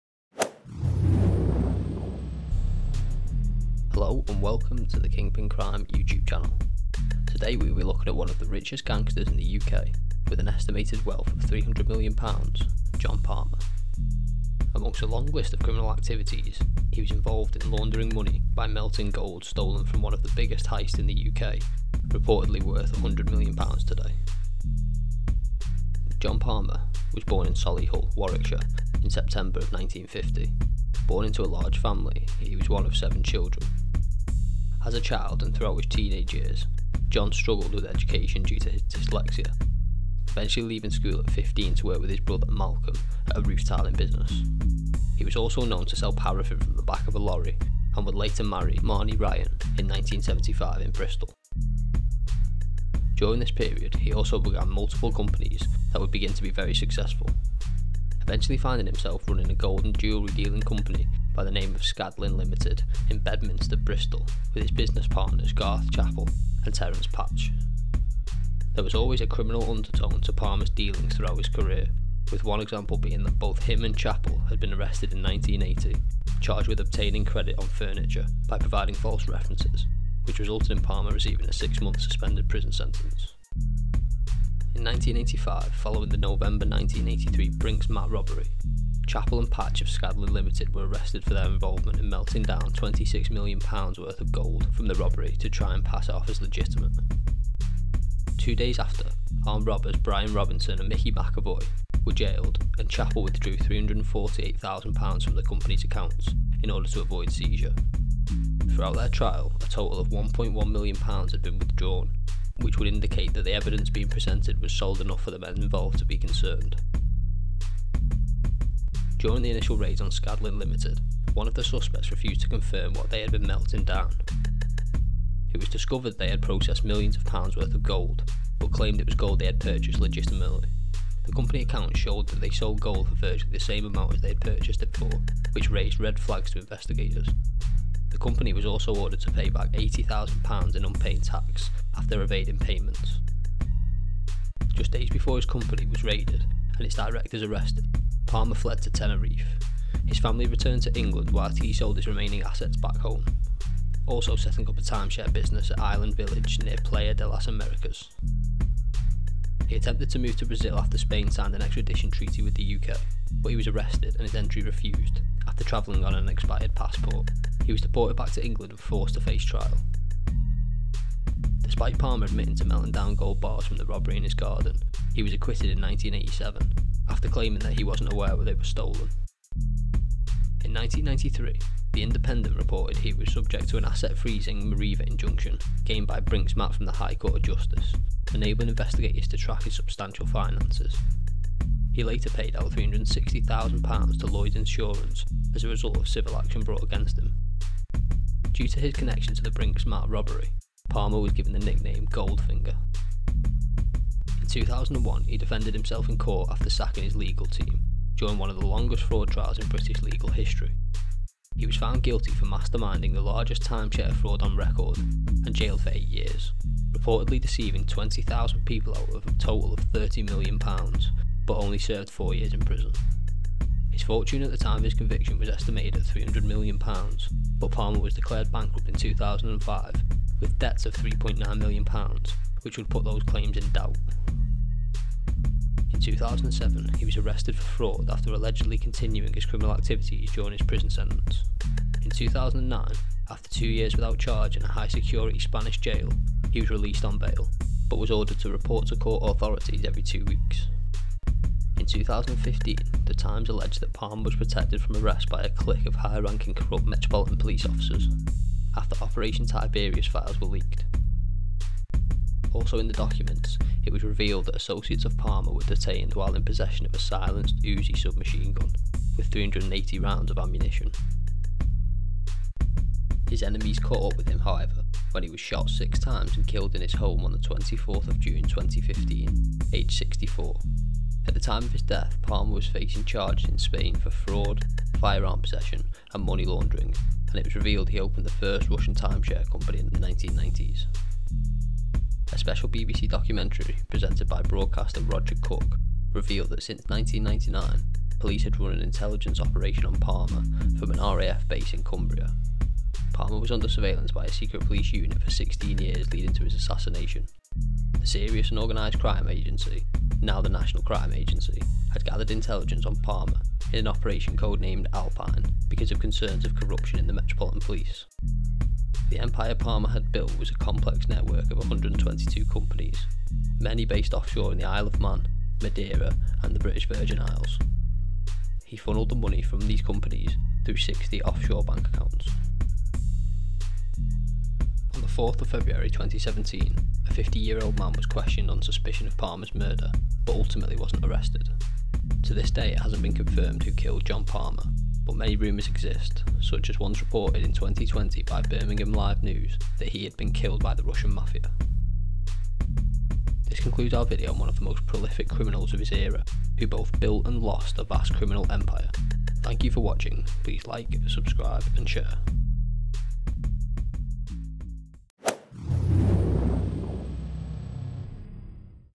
The British Kingpin Worth £300M - John Palmer or "Goldfinger"
The Story of the Man Dubbed "Britain's Richest Criminal" at his height, John Palmer would be involved in the aftermath of the biggest gold robbery of his era. He built a criminal empire, surviving multiple attempts on his life and fleeing England for Spain when the police closed in. He ended his career with a network of 122 off shore shell companies and 60 off shore bank accounts which helped him to legitimise the cash from his illegitimate business dealings.

Kingpin Crime